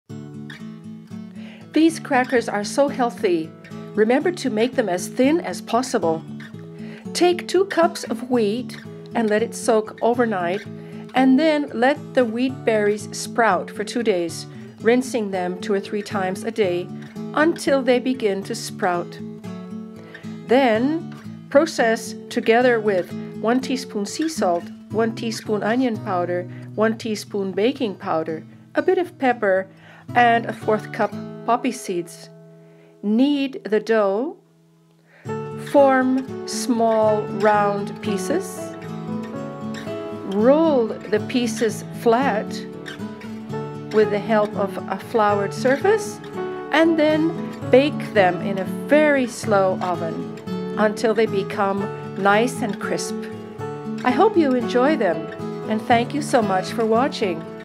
Sprouted Wheat Crackers - Galletas de brotes de trigo
Want something healthy, simple and crunchy?
(Subtitulos y receta disponibles en español)
SOAK 2 cups wheat berries in water overnight
RINSE them 3 times a day and allow to
SPROUT for 2 days
PROCESS
1 teaspoon seasalt
1 teaspoon onion powder
1 teaspoon baking powder
OPTIONAL 1/4 cup poppy seeds or sesame seeds
KNEAD the dough together
FORM small balls
ROLL out VERY THIN on a floured Surface
BAKE in a very low oven till completely crisp